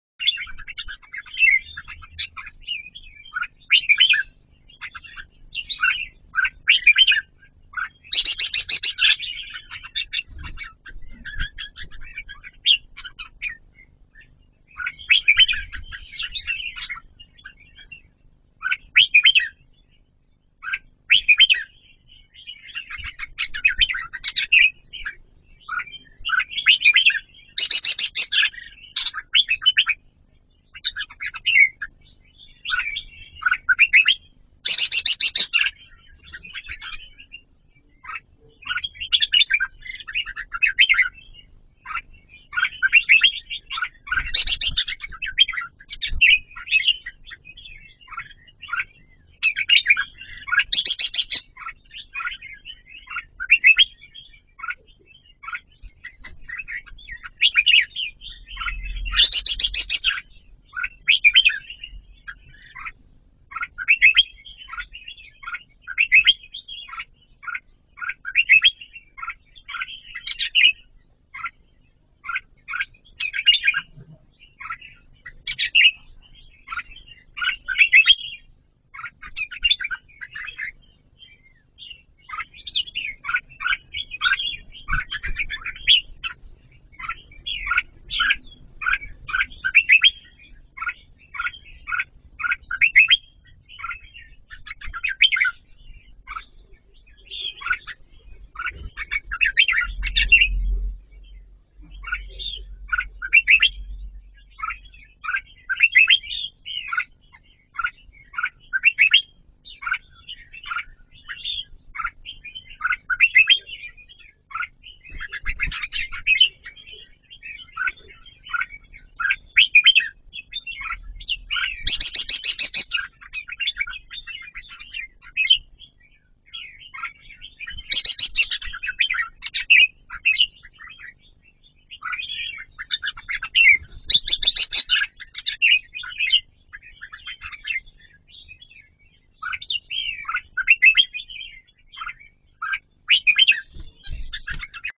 Theo dõi trực tiếp các CB Chào mào Bông Bạch Socola Ghép qua Camera//Chim Đẹp giọng hay
Nếu có thể, hãy Tham gia làm hội viên của kênh này để được hưởng đặc quyền: